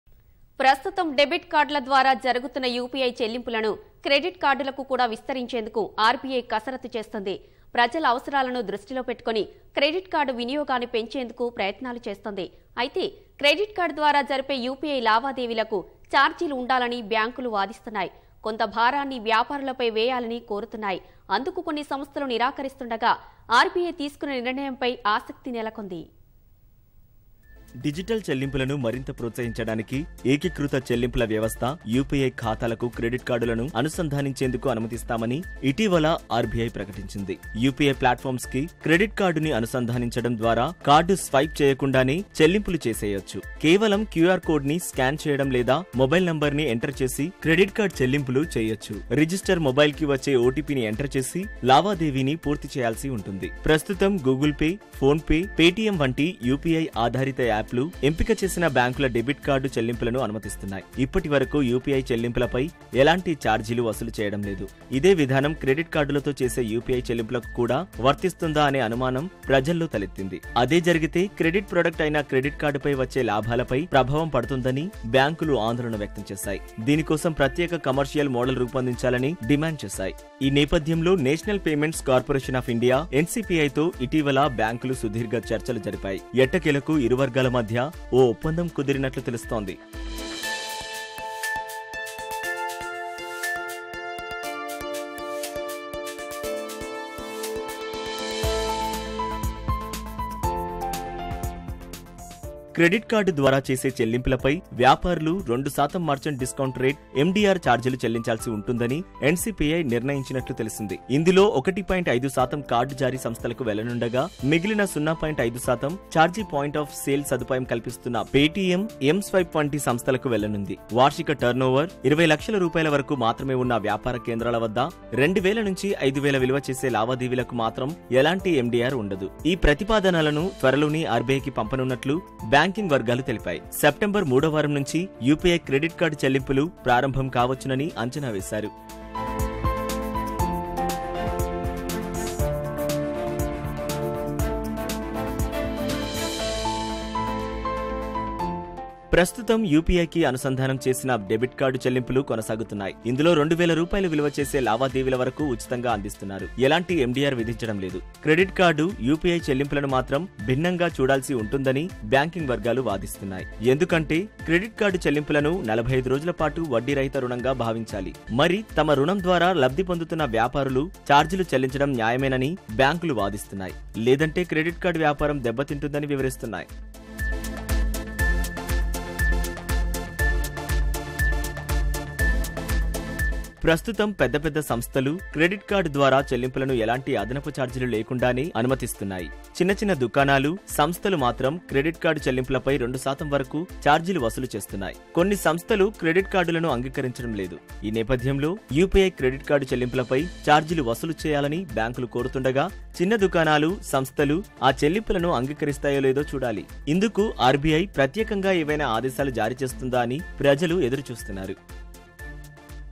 Credit Card on UPI: NPCI in Talks With Banks for Pilot | కార్డు స్వైప్‌ చేయకుండానే చెల్లింపులు
ప్రస్తుతం డెబిట్ కార్డుల ద్వారా జరుగుతున్న యూపీఐ చెల్లింపులను..క్రెడిట్ కార్డులకు కూడా విస్తరించేందుకు. RBI కసరత్తు చేస్తోంది. ప్రజల అవసరాలను దృష్టిలో పెట్టుకొని క్రెడిట్ కార్డు వినియోగాన్ని పెంచేందుకు. ప్రయత్నాలు చేస్తోంది. అయితే క్రెడిట్ కార్డు
ద్వారా జరిపే యూపీఐ లావాదేవీలకు. ఛార్జీలు ఉండాలని బ్యాంకులు వాదిస్తున్నాయి.
కొంత భారాన్ని వ్యాపారులపై వేయాలని. కోరుతున్నాయి. అందుకు కొన్ని సంస్థలు
నిరాకరిస్తుండగా...RBI తీసుకునే నిర్ణయంపై ఆసక్తి నెలకొంది.